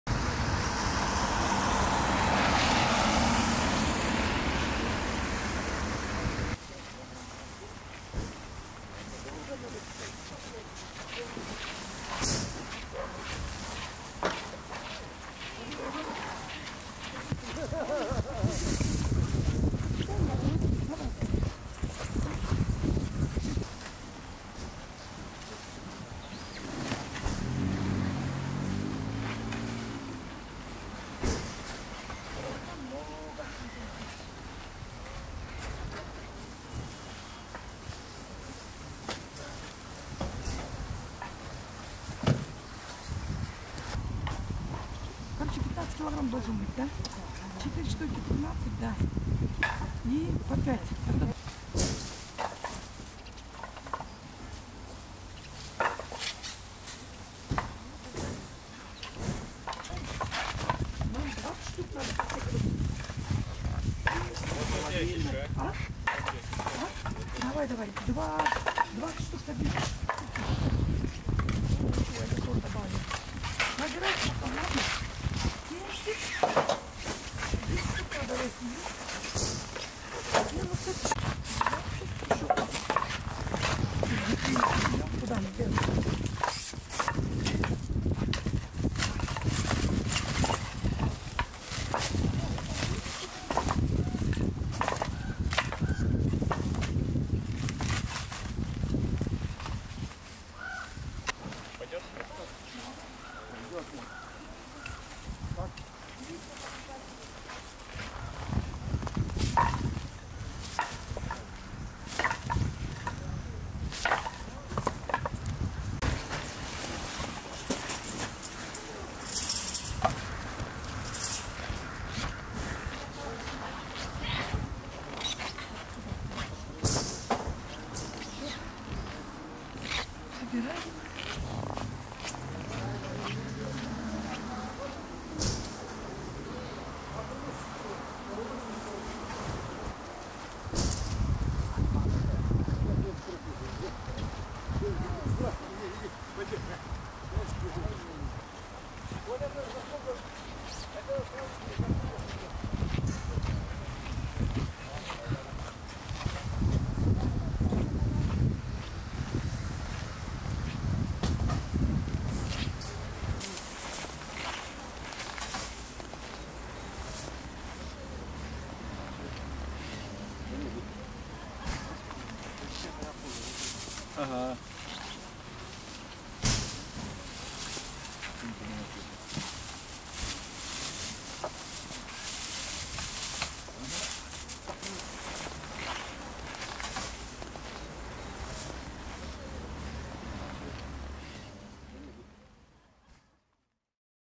Yakutsk: Shopping at the Coldest Fish Market on Earth
How about to do some shopping at the coldest fish market on Earth?
Enjoy this video from the Yakutsk Krestyansky outdoor fish and meat market. Yakutia (Sakha Republic), Siberia. February 2021.

4K footage of this video available on request.